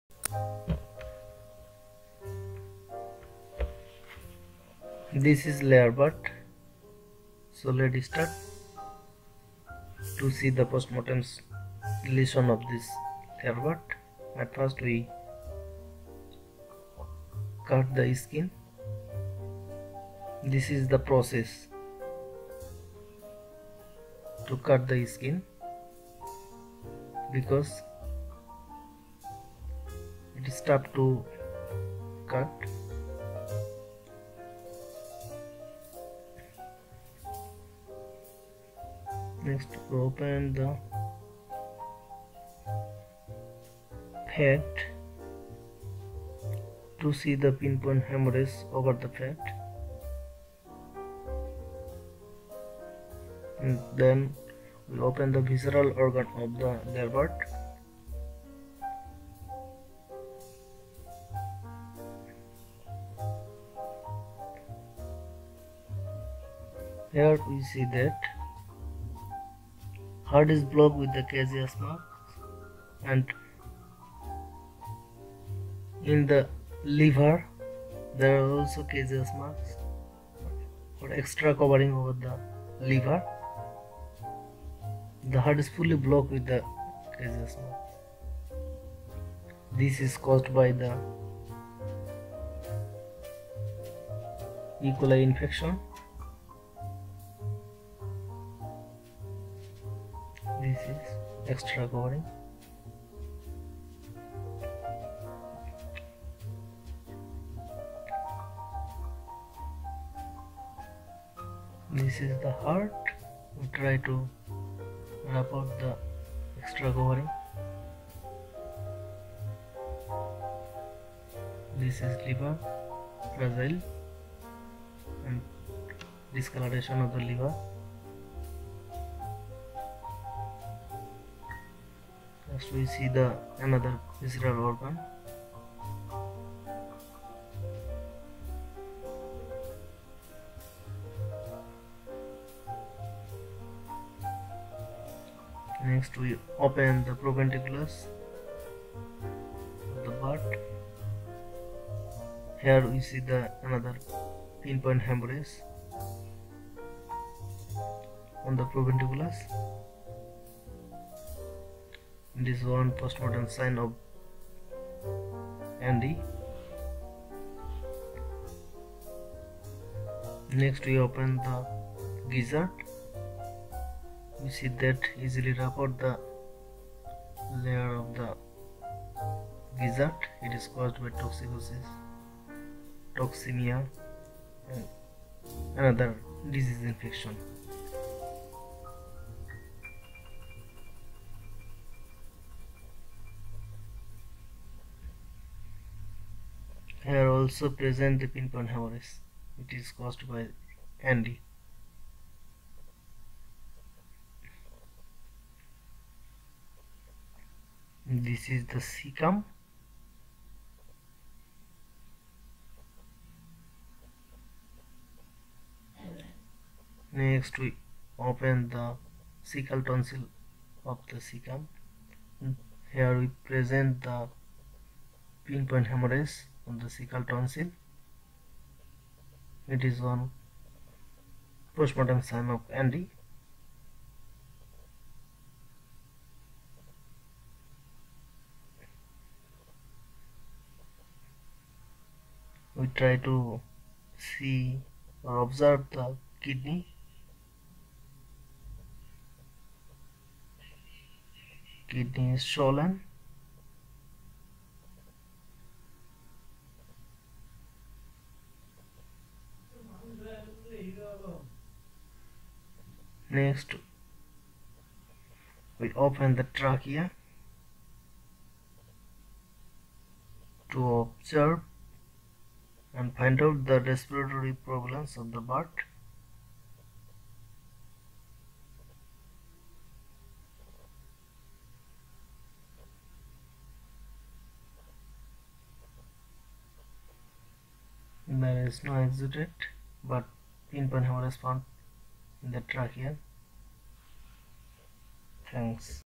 Postmortem lesions of Newcastle disease+FT+collibacilosis of layer bird
Signs(in case of mixed infection)

    Respiratory signs, coughing, sneezing.
    Snick.
    Dejection.
    Reduced appetite,
depression, watery greenish diarrhea, and swelling of the tissues of the head and neck are typical of the most virulent form of the disease

postmortem sign:
Airsacculitis.
Pericarditis.
Perihepatitis.
Swollen liver and spleen.
Peritonitis.
Salpingitis.
Omphalitis.
Synovitis
pin point hemorrhage on cecal tonsil
pin point haemorrhage in proventiculus and gizzard
etc